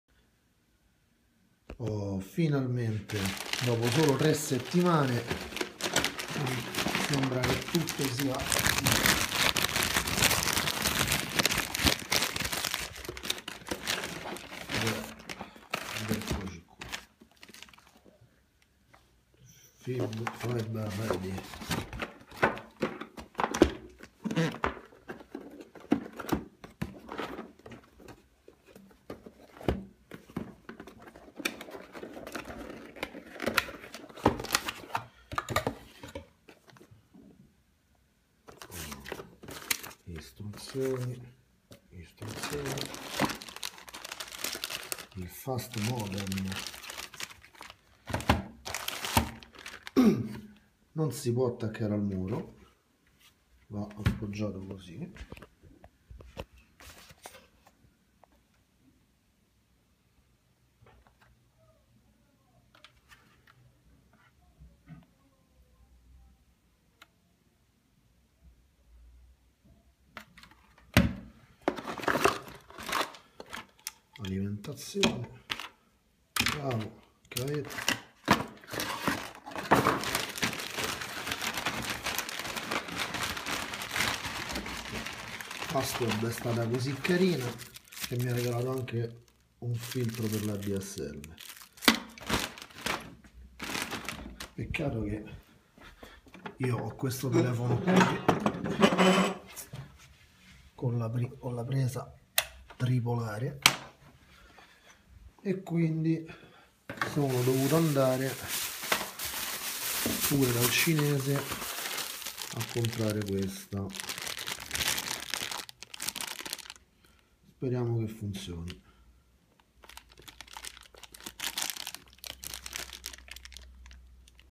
Modem Fastweb ADB DV2200 - Offerta JET Dopo solo 3 settimane il modem è arrivato
LINK AL PRODOTTO
Ho sottoscritto il contratto con Fastweb e "rapidamente" in 3 settimane hanno attivato i servizi . Dopo che il loro commerciale, prostituitosi telefonicamente, è riuscito a convincermi che la mia abitazione era rggiunta dall' Adsl a 20 mega (sono a roma) , ho verificato , dopo vari speed test che il primo giorno in download andavo a 19 mega ( su emule 1900 per capirci), quindi bene. Il secondo , un pò meno, il terzo a 11 . E 11 è la velocità minima garantita, se ti dicono che hai l'adsl a 20 mega.
Poi, vado a vedere i servizi telefonici attivabili:
visualizzazione del numero che chiama + 1€
segreteria telefonica + 1€
blocco dei cellulari + 1€
fax virtuale a pagamento.. e così via..
Riepilogando , JET i primi 12 mesi costa 25 euro + eventuali chiamate , che sono a consumo. Se aggiungessi il blocco chiamate vs cellulari, il chi è e se attivassi il fax .. alla fine mi sono reso conto che tutto sommato.. quasi quasi mi conveniva fare alice... che ha messo la fibra ottica proprio sull'armadietto dove sono allacciato...
Vedermo
P.s.
secondo me il modem Technicolor TG589vn v2  , andava meglio